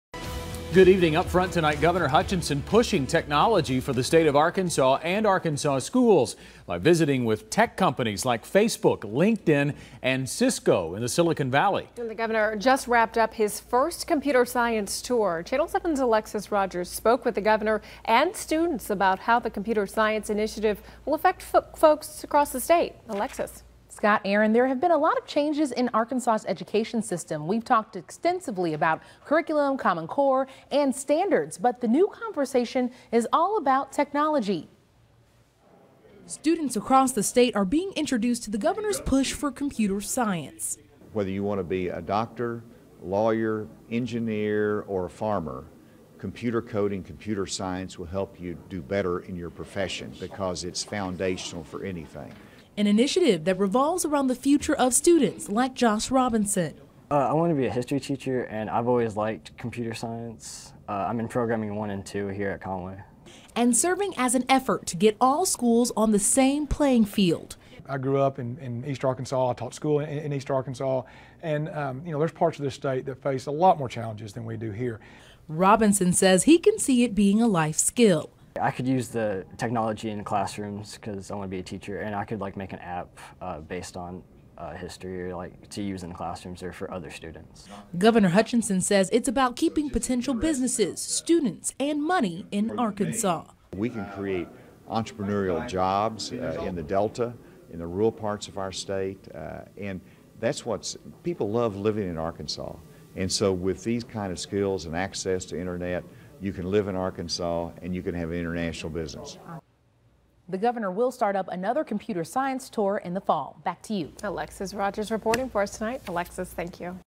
Governor pushes technology, computer science in Arkansas
Governor Hutchinson is pushing technology for the state of Arkansas and Arkansas schools by visiting with tech companies like Facebook, LinkedIn and Cisco in Silicon Valley. The governor recently wrapped up his first computer science tour.

Students across the state are being introduced to the governor's push for computer science.

“Whether you want to be a doctor, lawyer, engineer or a farmer...computer coding, computer science would help you do better in your profession. Because it's foundational for anything,” Hutchinson said.

The governor's initiative revolves around the future of students like Conway High sophomore Josh Robinson.

"I want to be a history teacher. I've always like computer science. I am in programming one and two here at Conway,” Robinson said.

Joel Linn is the principal of Conway High School. He said he sees a benefit in having robust computer science programs in schools.

"I grew up in east Arkansas, I taught school in east Arkansas and there's parts of this state that face much more challenges than we do here,” Linn said.

Robinson said he can see it being a life skill.

“I could use the technology in classrooms because I want to be a teacher and I could make an app based on history to use in the classroom or for other students,” Robinson said.

Hutchinson told Channel 7 News the initiative is about keeping potential businesses, students, and money in Arkansas.

"We can create entrepreneurial jobs in the Delta, in the rural parts of our state. And that's what people love living in Arkansas. So with these types of skills and access you can live in Arkansas and have international business,” Hutchinson said. "Students have choices. There's a lot of competition there. It might not be for everybody but if we could have 20% of our students taking computer science then we are going to be able to grow our economy in this state. Really in every field."

The governor will conduct another computer science tour in the Fall.